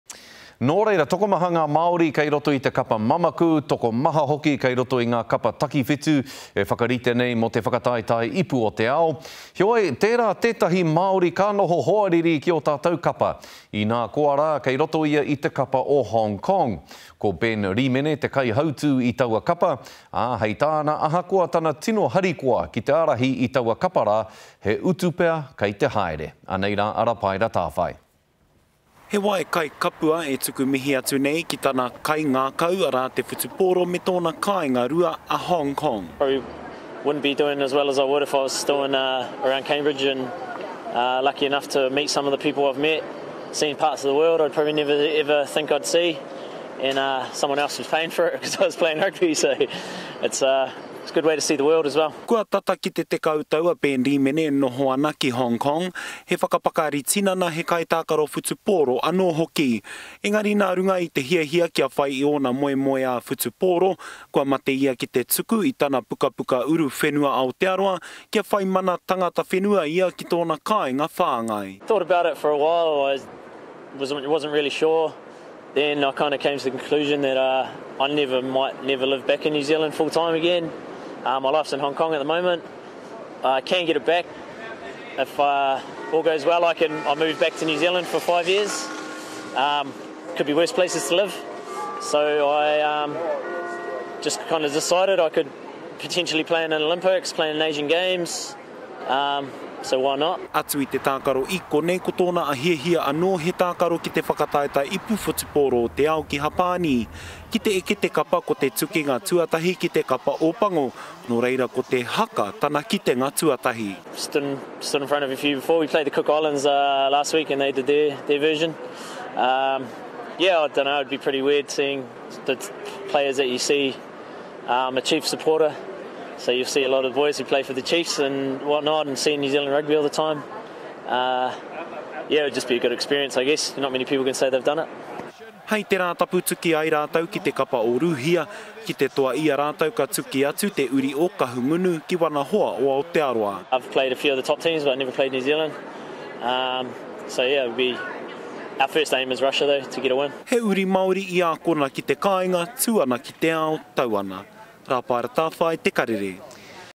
Rangitāne’s Ben Rimene to captain Hong Kong Sevens team
While our sevens teams prepare for their first games another Māori is also preparing for his first hit out but for his adopted country Hong Kong. Ben Rimene who also happens to be the captain for his side says its a great opportunity but may come with a price. Rapaera Tawhai with more.